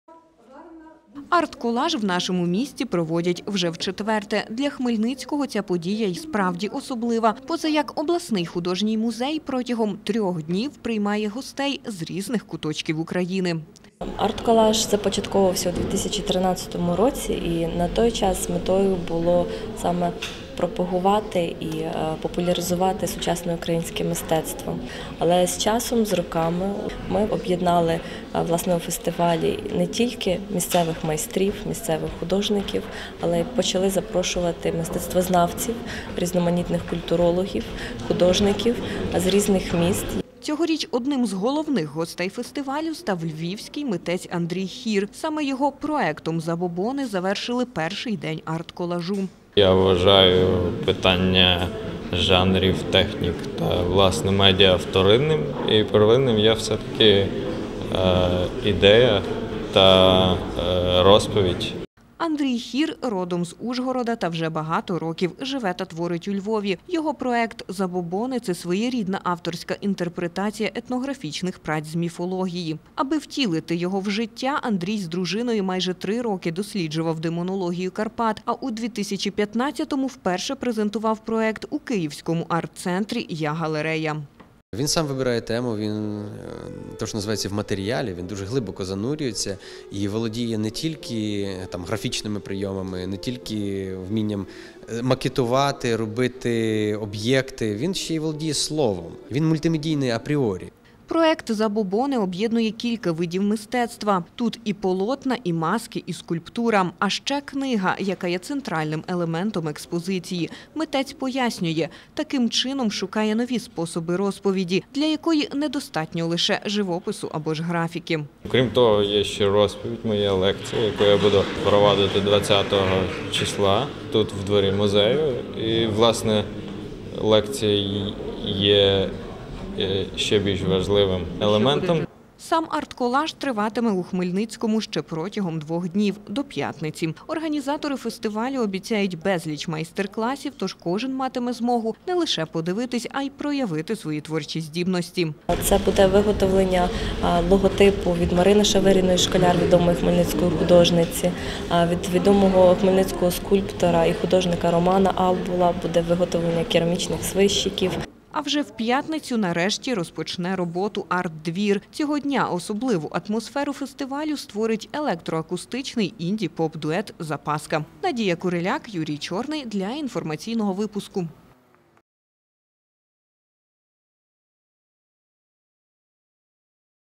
Атр колаж у Хмельницькому
Хмельницький художній музей святкує тридцятиріччя. А ще – зустрічає гостей на щорічному фестивалі «АРТ-колаж». У рамках проекту об’єднують різні види мистецтва – від живопису до музики. Його головна мета – розвивати сучасне українське мистецтво та зацікавити якомога більше  людей, навіть тих, хто зазвичай на музеї часу не знаходить.